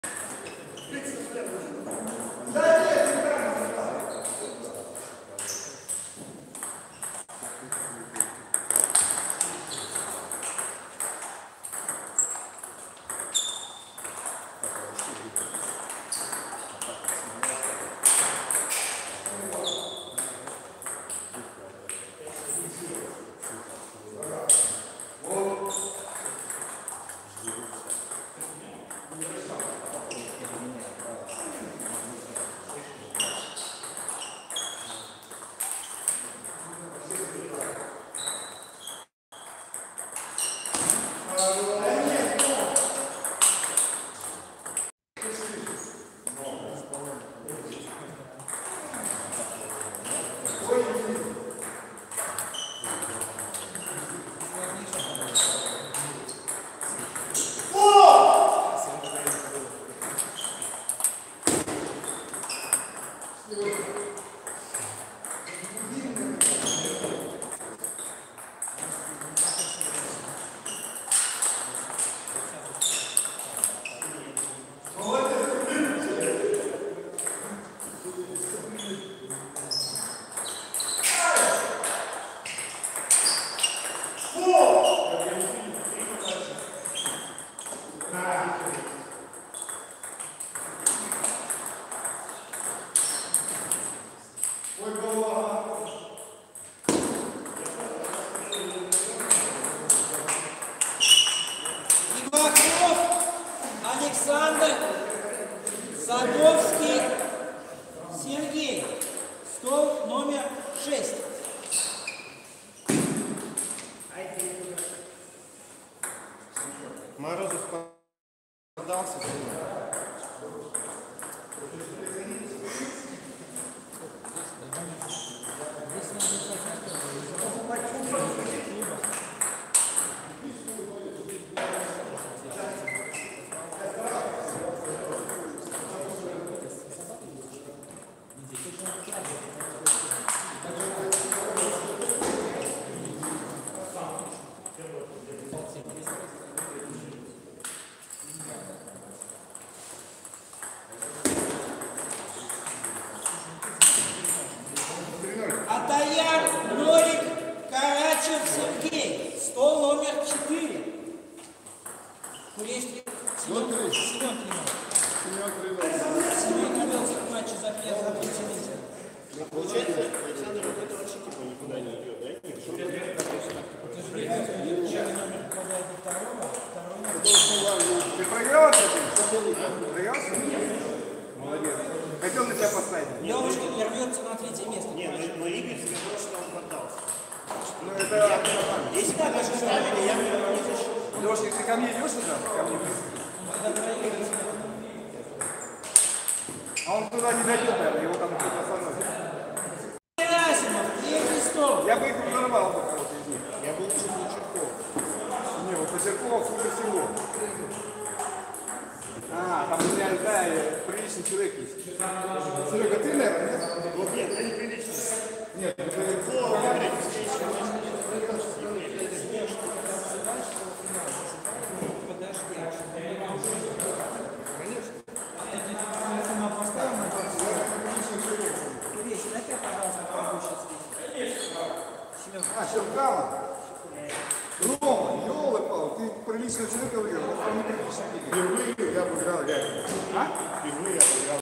настольный теннис Кустов П. - Тюхлов И. турнир в ТТКЛУБ 20.02.2020 Санкт-Петербург
Наша группа vk ttplayspbcom